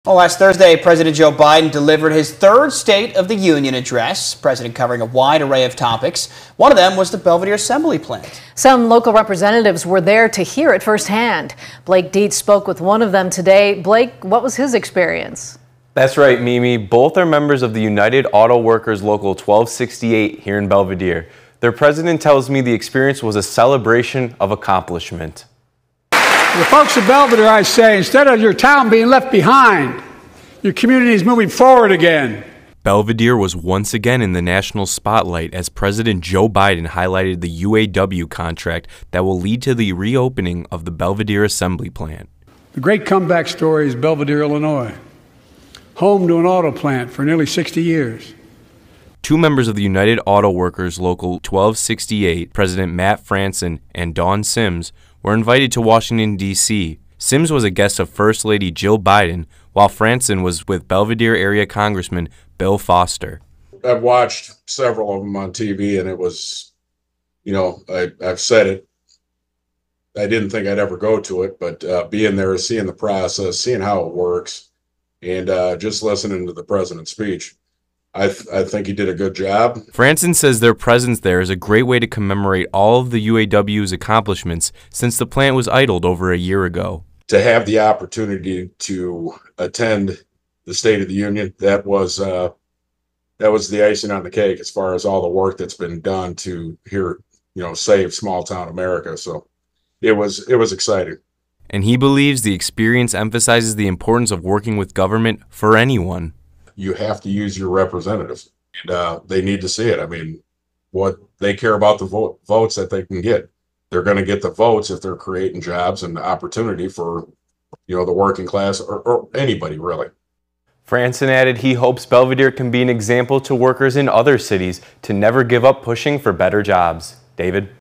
UAW reps went to the State of the Union address after contract was signed to reopen Belvidere assemb
Local representatives of the United Auto Workers said Biden's mention of Belvidere's Stellantis Assembly Plant in the State of the Union address was a cause for celebration.